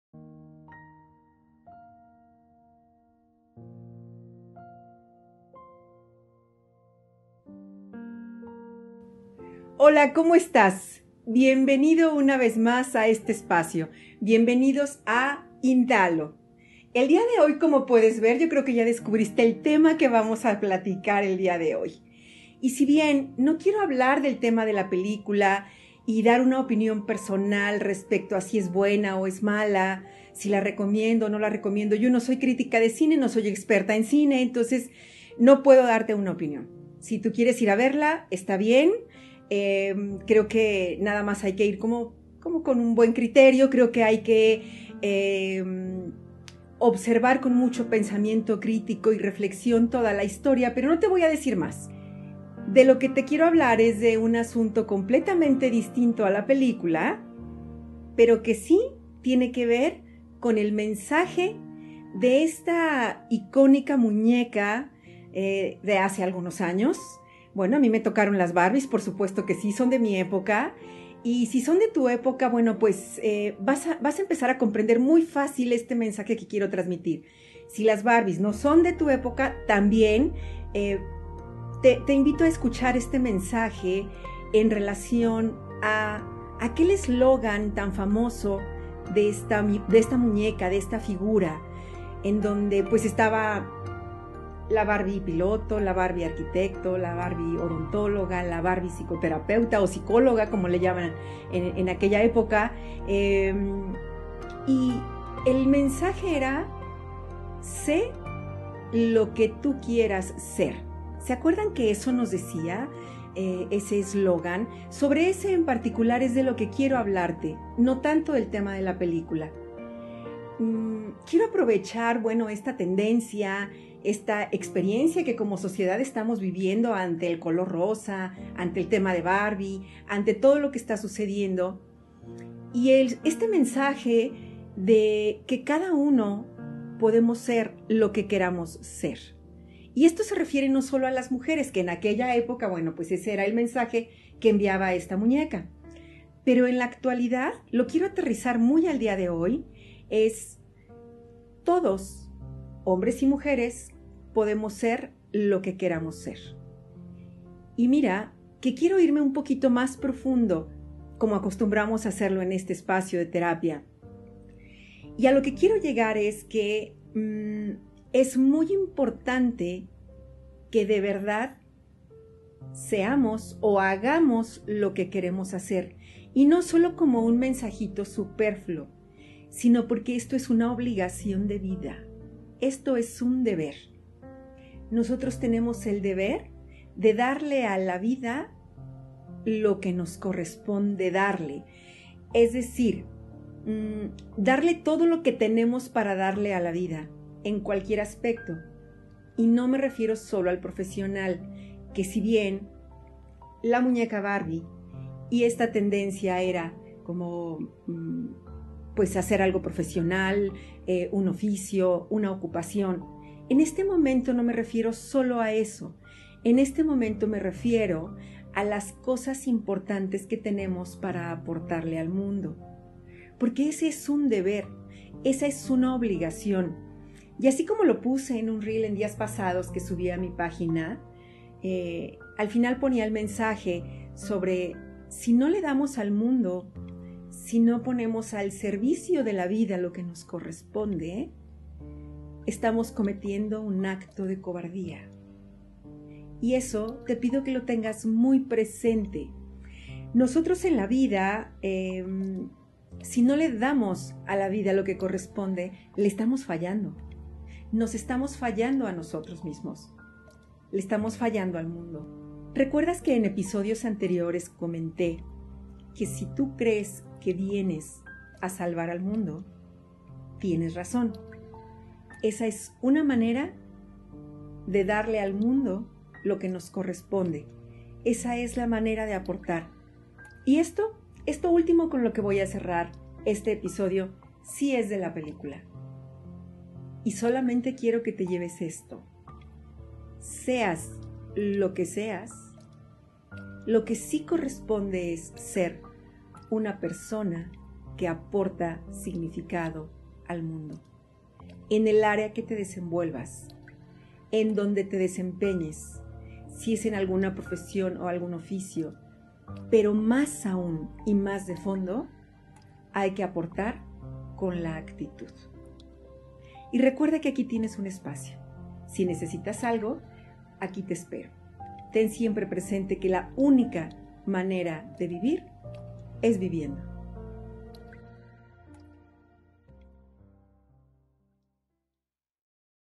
INDALO – T1 – E9- Barbie “sé lo que tú quieras ser”
Ser lo que queremos ser, es más que un gusto, un deber. Poner al servicio de la vida lo que tenemos para darle, es lo que nos corresponde como personas, si no, nos estamos fallando y a la vida también.  No siempre es en lo profesional o laboral, a veces es en actitud.

¡Gracias por tu confianza!

¡𝕽𝖊𝖈𝖚𝖊𝖗𝖉𝖆 𝖖𝖚𝖊 𝖑𝖆 ú𝖓𝖎𝖈𝖆 𝖒𝖆𝖓𝖊𝖗𝖆 𝖉𝖊 𝖛𝖎𝖛𝖎𝖗, 𝖊𝖘 𝖛𝖎𝖛𝖎𝖊𝖓𝖉𝖔!